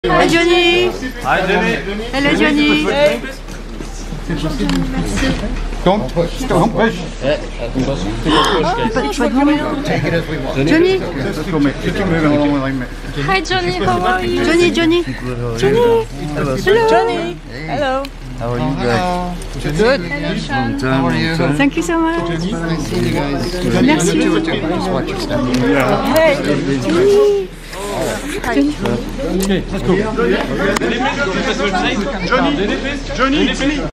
Johnny Depp Signs Autographs For Fans While Leaving The Pantheon Cinema In Paris, France
JohnnyDepp Signs Autographs For Fans While Leaving The Pantheon Cinema In Paris, France - TheHollywoodFix.net
[Video & Imagery Supplied By KCS Presse/MEGA]
©️ (Copyright TheHollywoodFix)